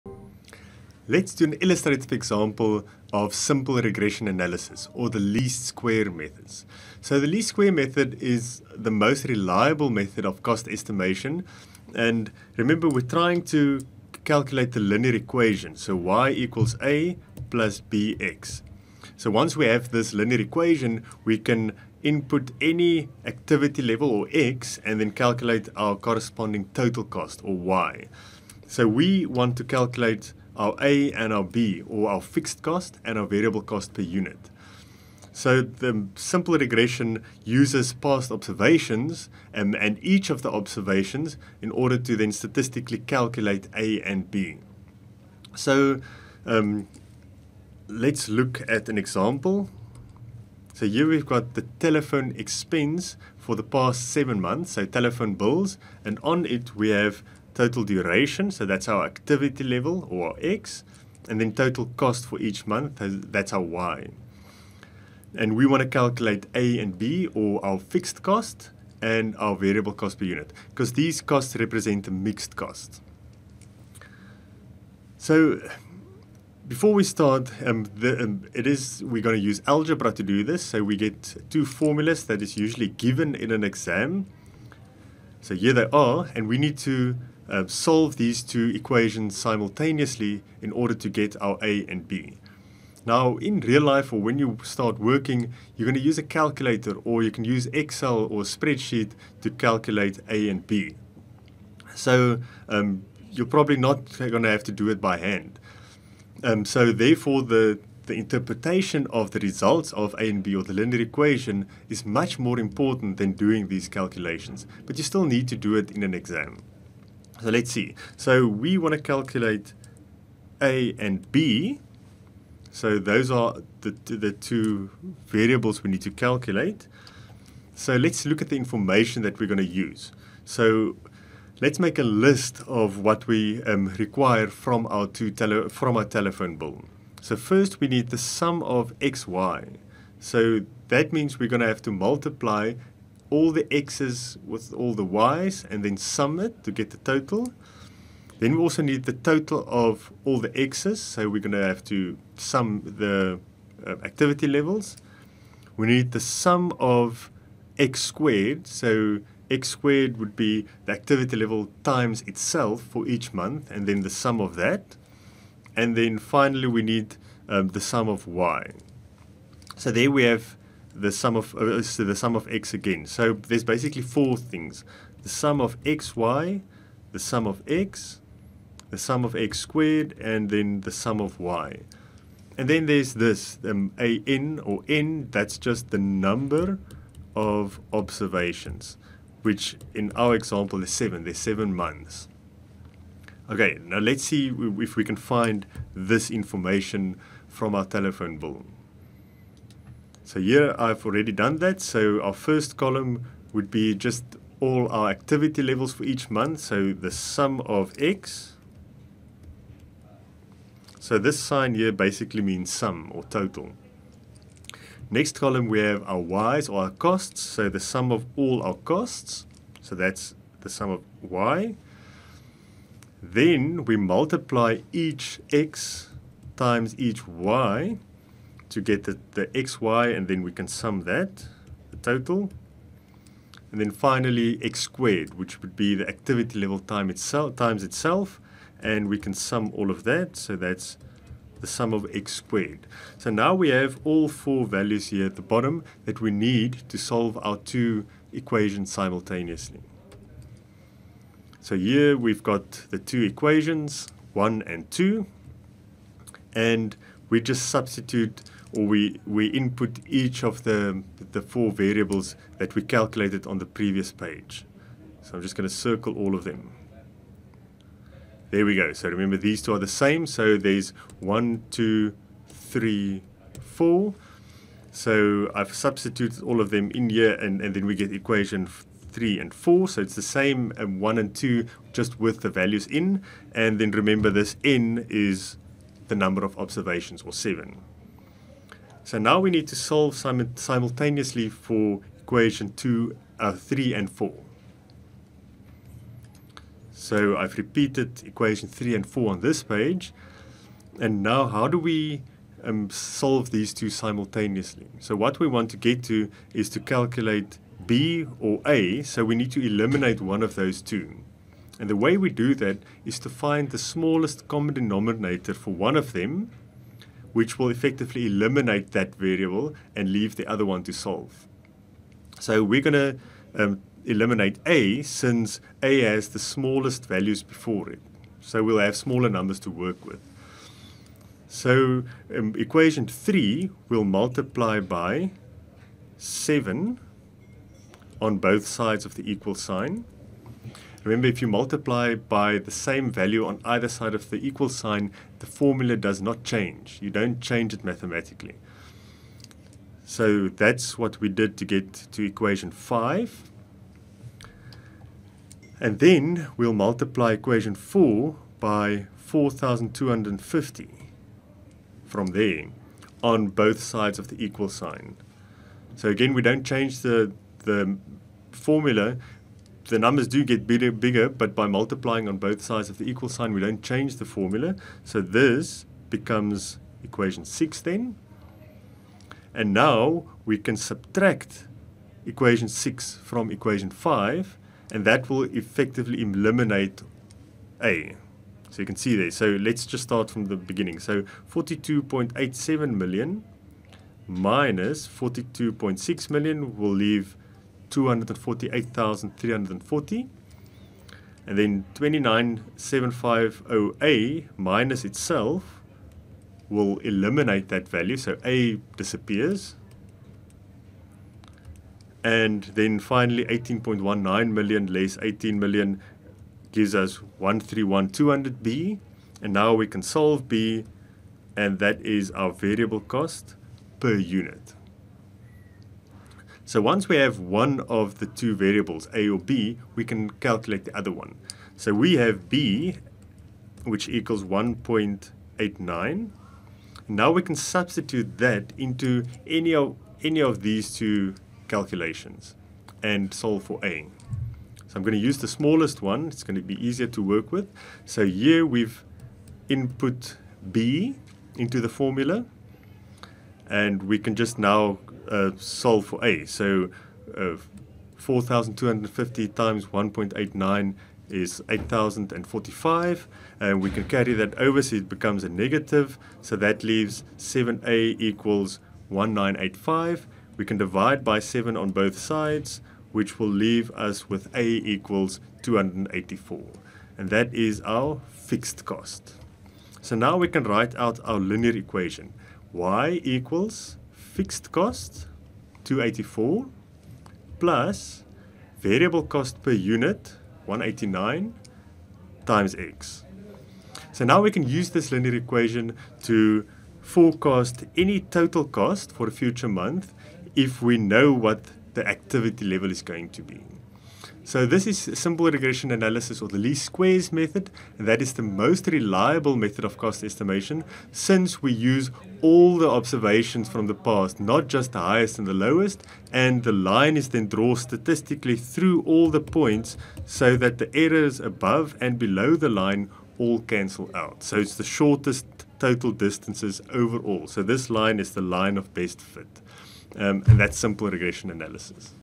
Least Squares Regression Method (9 of 9) | LU4: Cost Estimation (MAC2601)
UNISA MAC2601 Learning Unit 4: Cost Estimation - Lecture 9 of 9

In this video we introduce the basics of the least squares regression method as a tool to estimate costs in management accounting.

For more info on our support courses, check out this video